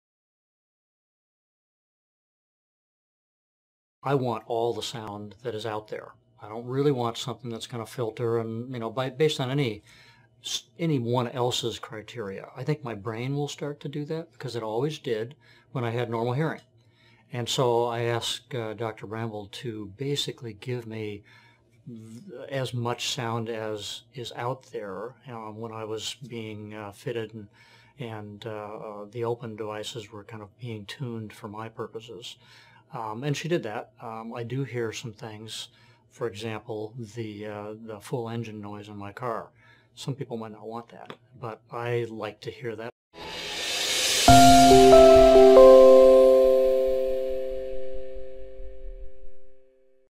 Oticon Opn™ user | Chris: ”I want all the sounds that’s out there”
Hear how Oticon Opn and BrainHearing technology help Chris get all the sound in his life back including the engine noise.